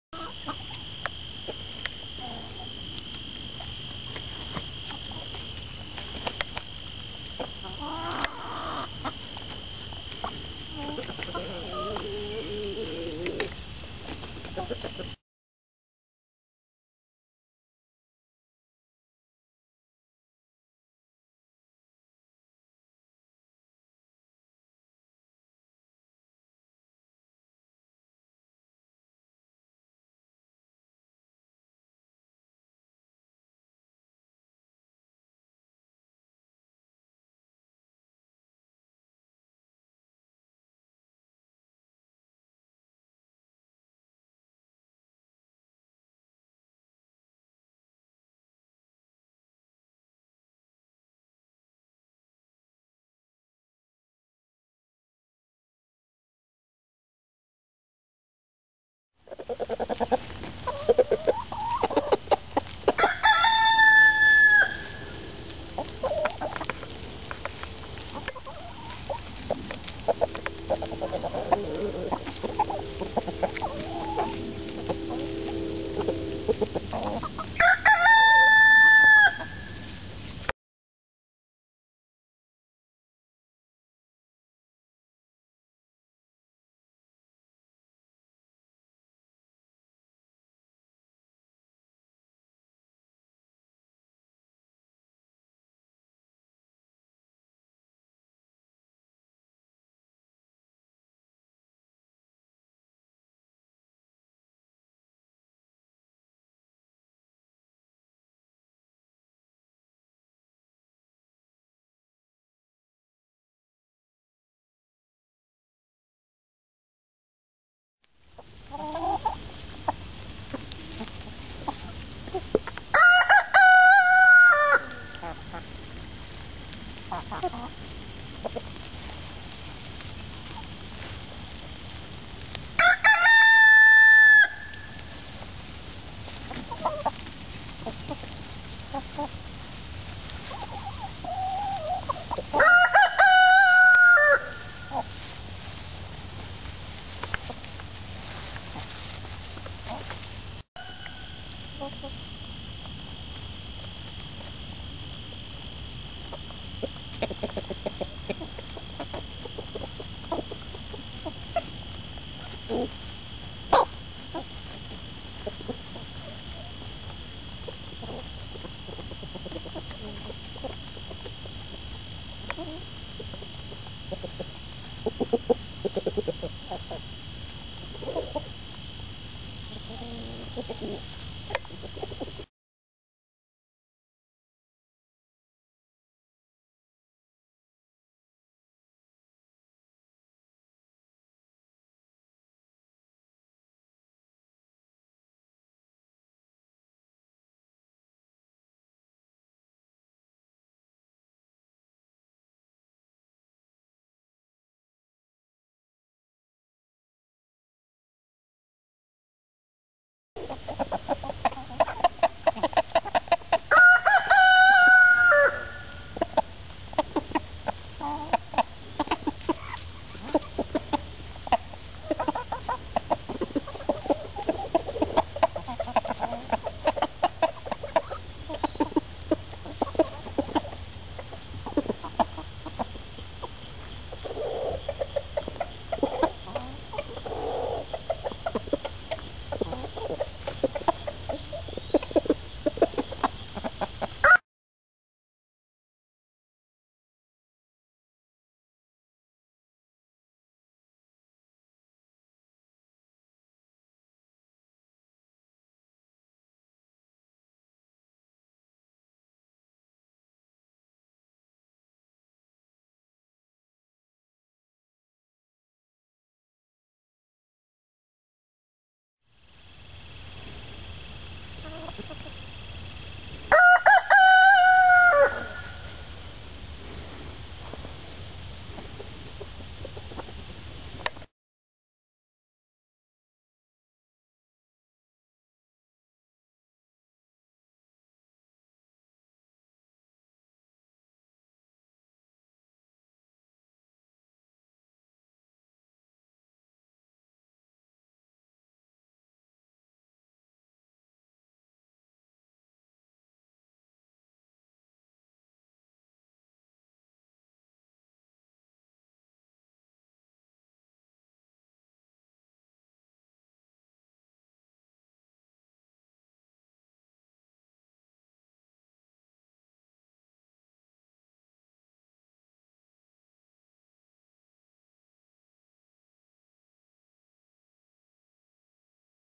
Snowdrop the Turkochin
My turken/cochin mix hen, Snowdrop.

Yes I'm aware of the gaps, sorry about that.